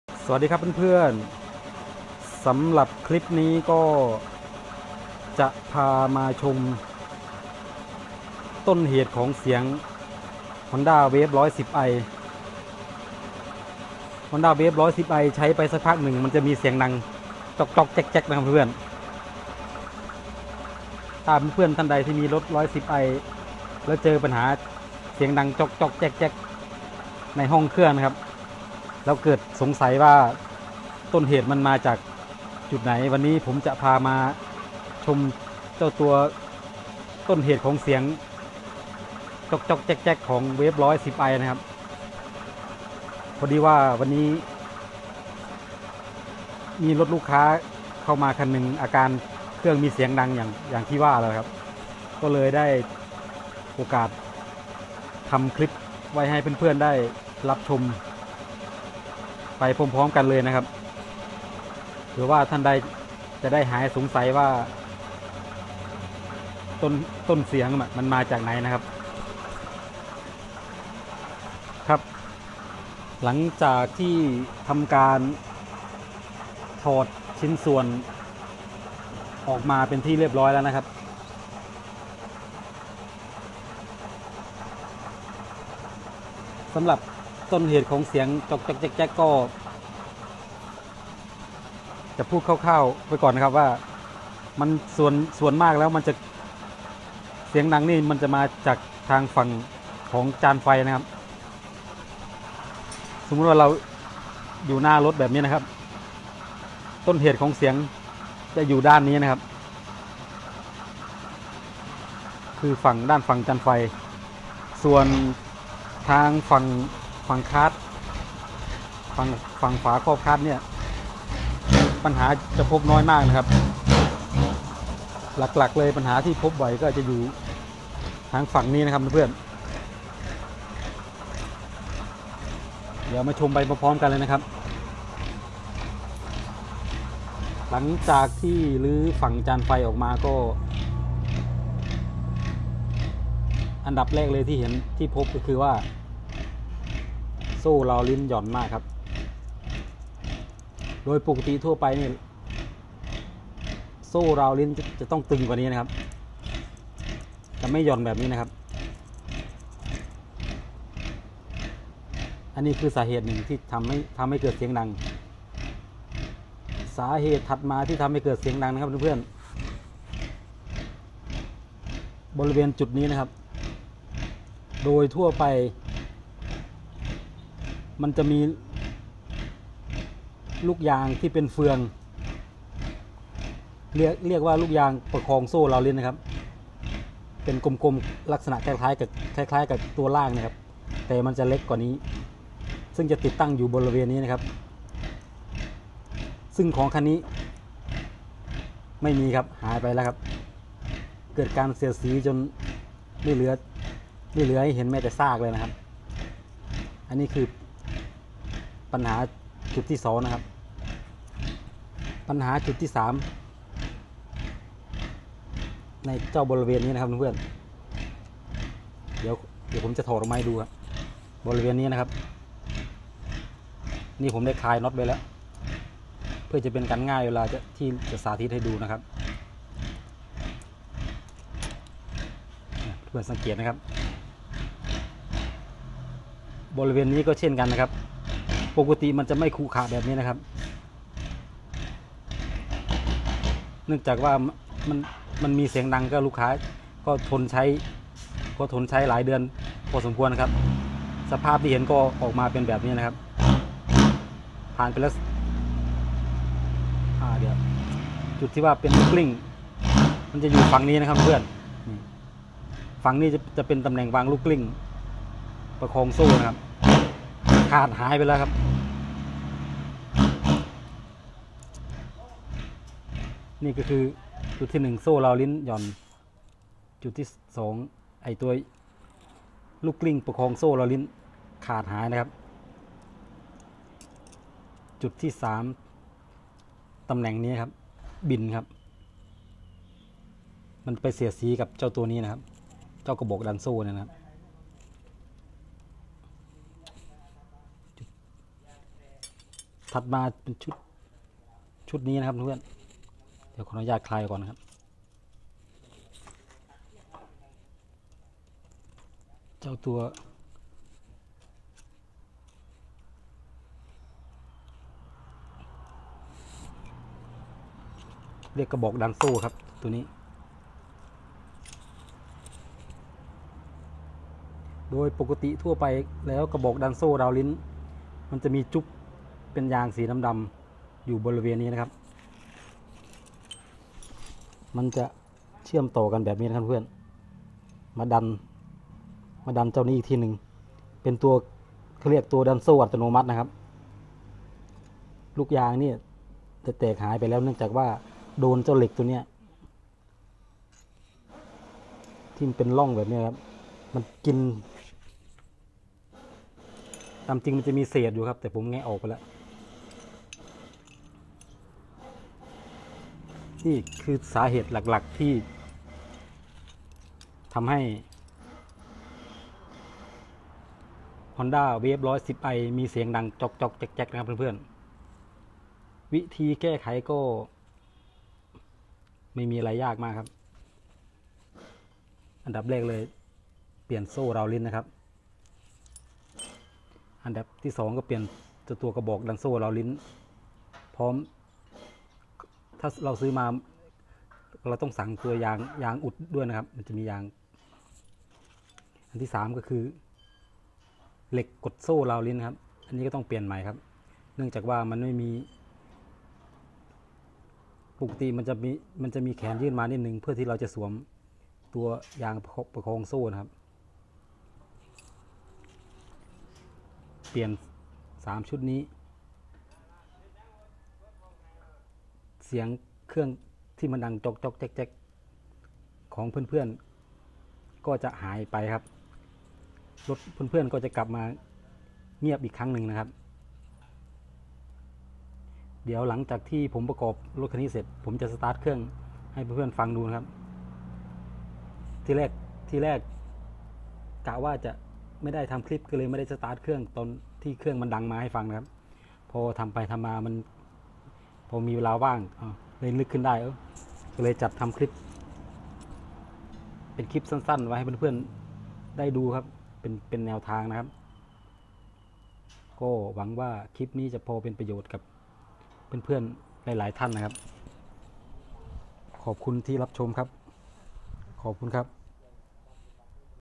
พาชม ต้นเหตุที่ทำให้เวฟ110 ไอมีเสียงดังจ๊อกๆแจ็คๆ
ติดตามเพจ พาชม ต้นเหตุที่ทำให้เวฟ110 ไอมีเสียงดังจ๊อกๆแจ็คๆ@JAISERVICE สนใจอะไหล่จักยานยนต์ ติดต่อ